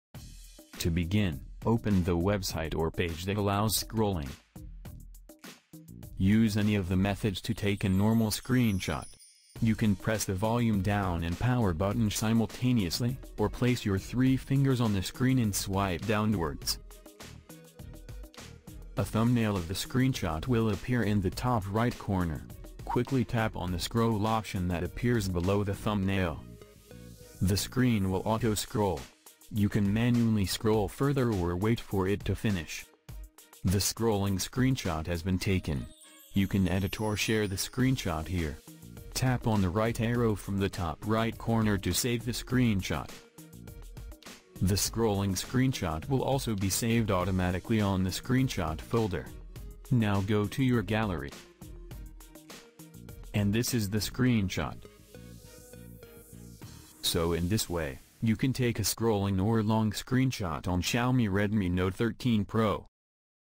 How to Take a Scrolling / Long Screenshot on Xiaomi Redmi Note 13 Pro
In this video I will show you, How to Take a Scrolling / Long Screenshot on Xiaomi Redmi Note 13 Pro.